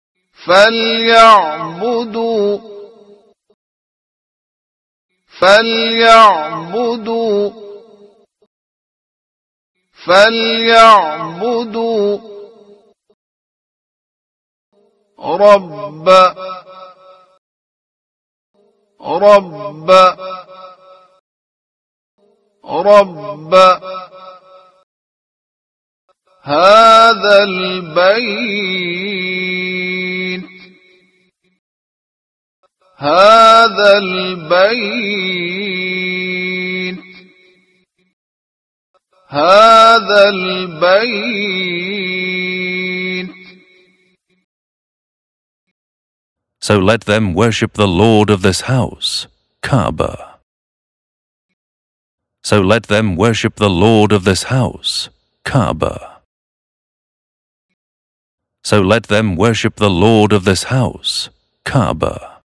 learning beginner quran with native english translation: vesre 3 Surah quraish
learning beginner quran and education word by word and repeat 3 times and with native english translate surah quraish verse 3